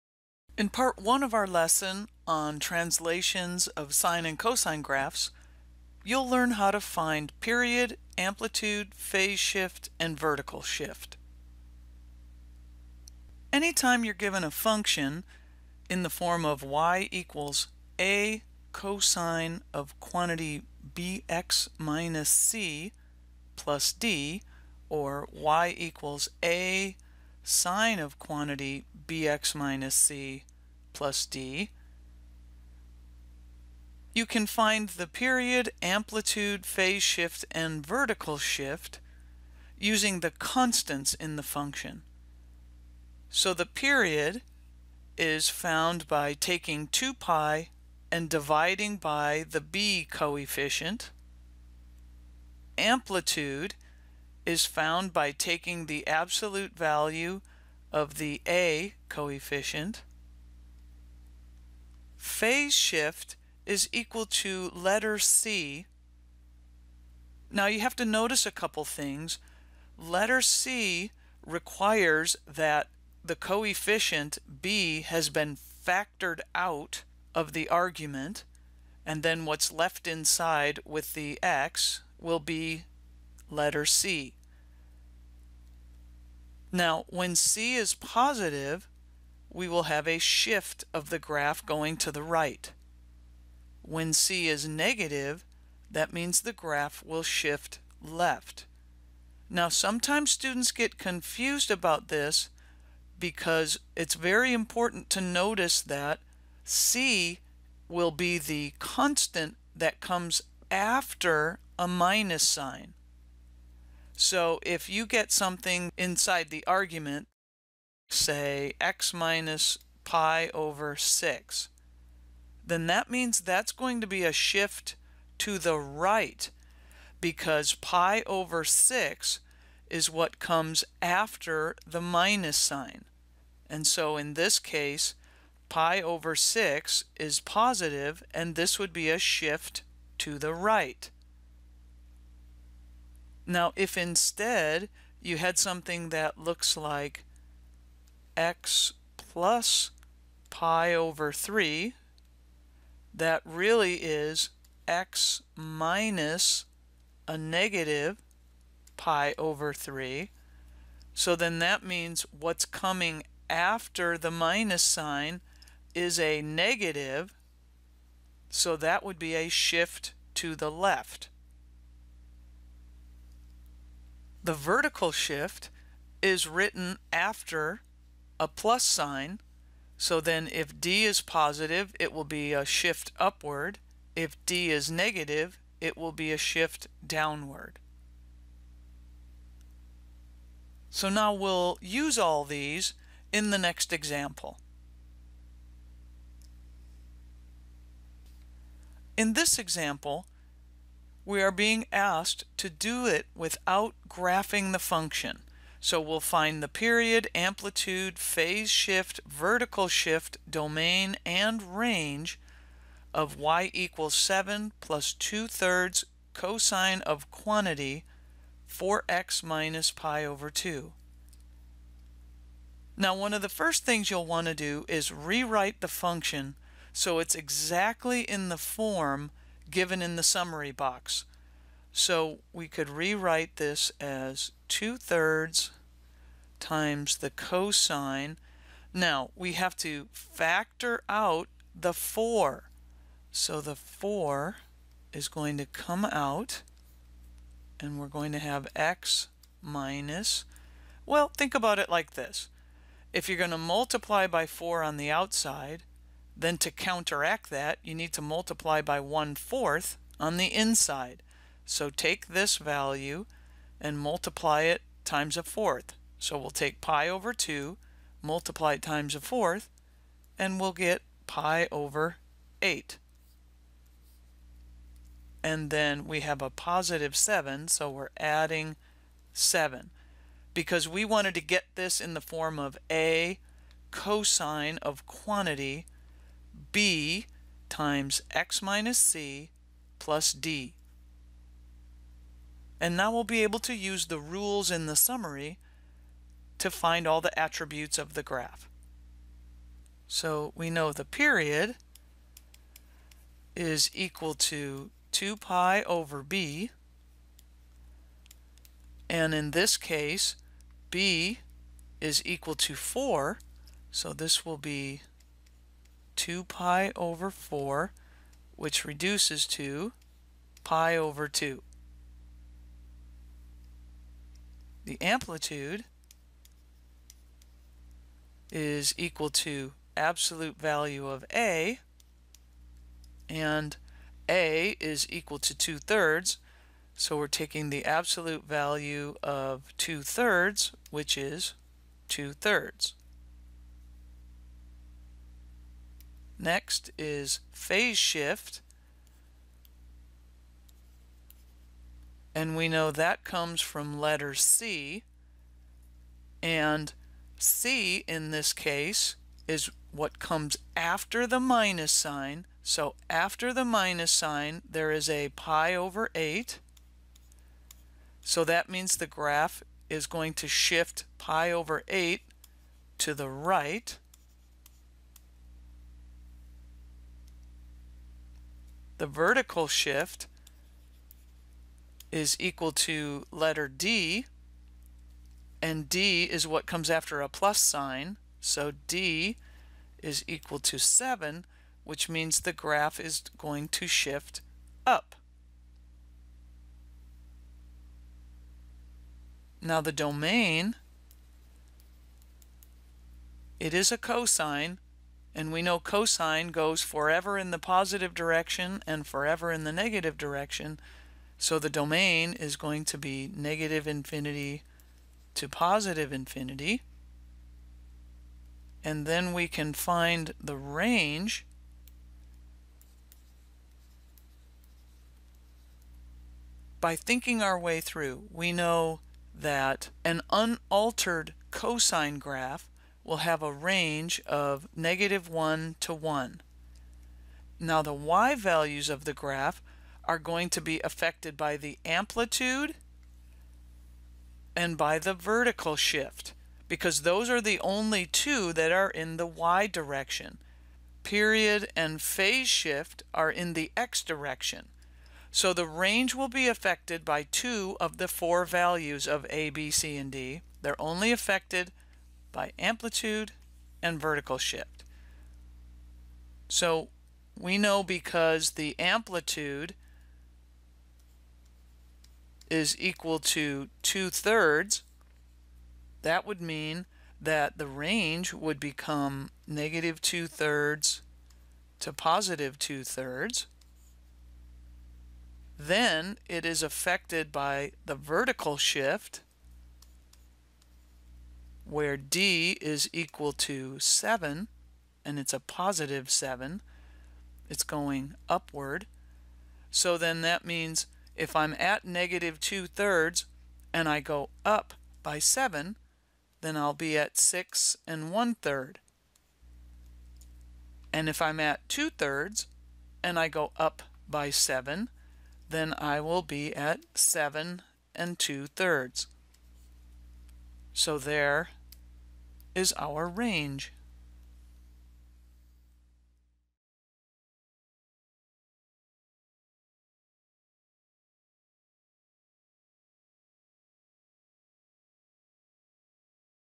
Math 142, 4.2 Part 1, Period, Amplitude, Phase Shift, and Vertical Shift of Sine and Cosine
Determine the Period, Amplitude, Phase Shift, and Vertical Shift of a Sine or Cosine function, without the graph.  Find the Domain and Range without the graph.